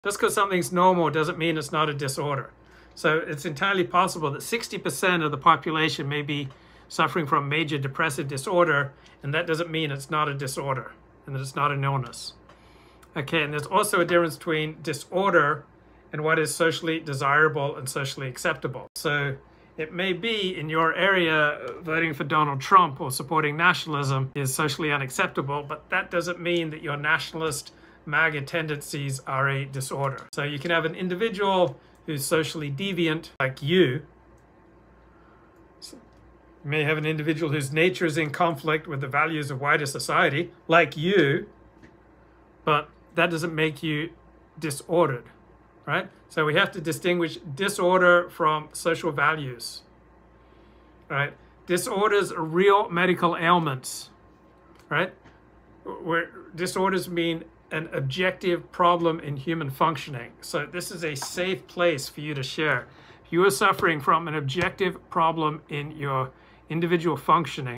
Embrace Your Individuality and Break Free from Social Norms💥
Discover the difference between disorder and societal acceptance. Join us as we explore the concept of individualism and the impact of social values. Let's celebrate uniqueness and find acceptance in our authentic selves.